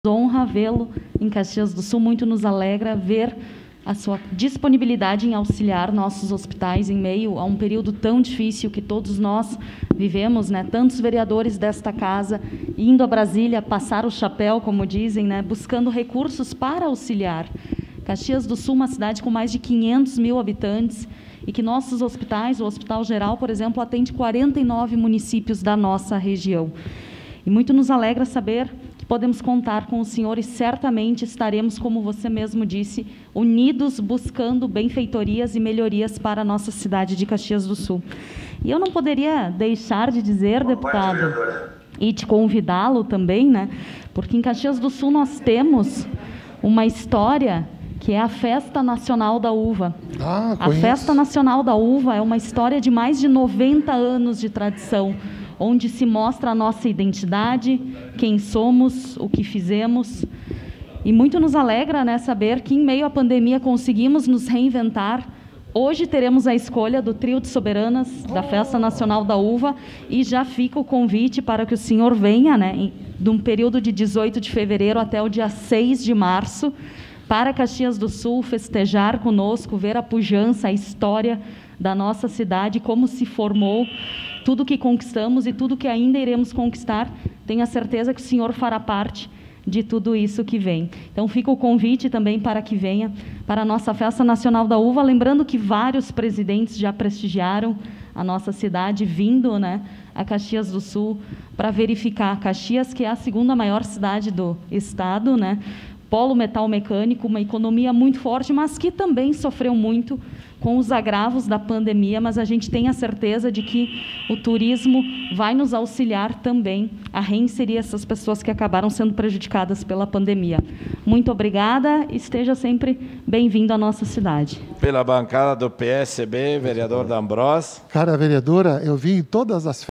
08 Tatiane Frizzo/PSDB na participação especial do Deputado Paulo Caleffi/PSD
A vereadora agradece a presença do deputado na sessão do dia e também agradece o apoio que ele da aos hospitais da cidade e da região da serra.